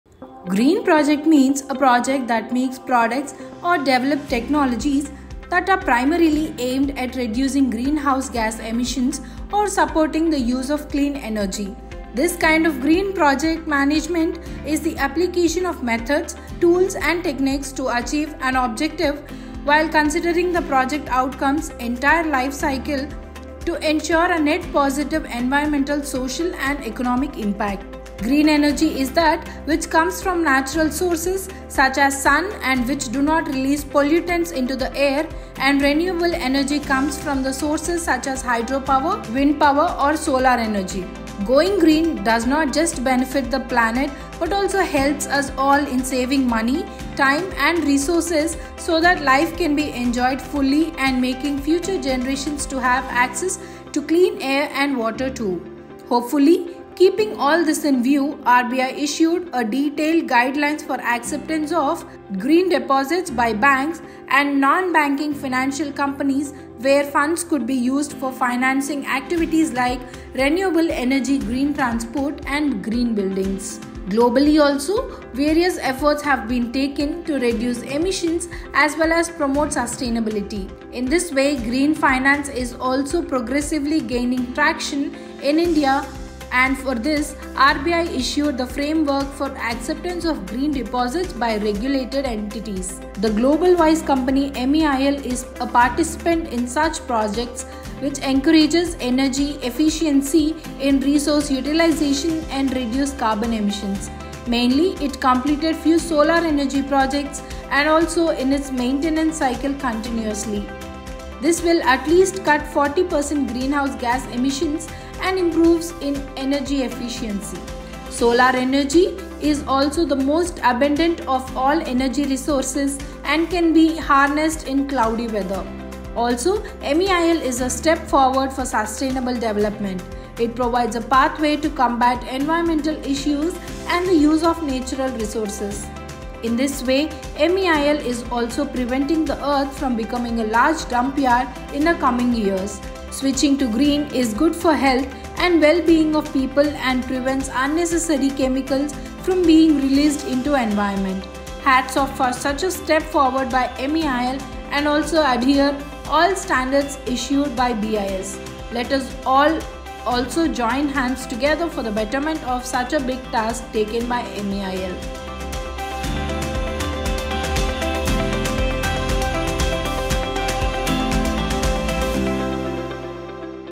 GREEN PROJECTS AND USE OF GREEN DEPOSITS || MEIL
Green project means a project that makes products or develops technologies that are primarily aimed at reducing green house gas emissions or supporting the use of clean energy.
This kind of green project management is the application of methods, tools and techniques to achieve an objective while considering the project outcomes entire life cycle to ensure a net positive environmental social and economic impact.
Green energy is that which comes from natural sources such as sun and which do not release pollutants into the air and renewable energy comes from the sources such as hydro power, wind power or solar energy.
Going green does it just benefit the planet but also helps us all in saving money, time and resources so that life can be enjoyed fully and making future generations to have access to clean air and water too.
Hopefully keeping all this in view RBI issued a detailed guidelines for acceptance of if green deposits by banks and non banking financial companies where funds could be used for financing activities like renewable energy green transport and green buildings.
Globally also various efforts have been taken to reduce emissions as well as promote sustainability.
In this way green finance is also progressively gaining traction in India and for this RBI issued the frame work for acceptance of green deposits by regulated entities.
The global wise company MEIL is participants in such projects which encourages energy efficiency in resource utilization and reduce carbon emissions. Mainly it completed few solar energy projects and also in its maintenance cycle continuously.
This will at least cuts 40% in greenhouse gas emissions and improves in energy efficiency. Solar energy is also the most abundant of all energy resources and can even be harnessed in cloudy weather. Always MEIL is a step forward for sustainable development. It provides a pathway to combat environmental issues and the use of natural resources.
In this way MEIL is also preventing the earth from becoming a large dump yard in the coming years.
Switching to green is good for health and well being of people and prevents unnecessary chemicals from being released in to environment. Hats off for such a step forward by MEIL and also adhere all standards issued by BIS.
Let us all also join hands together for the betterment of such a big task taken by MEIL.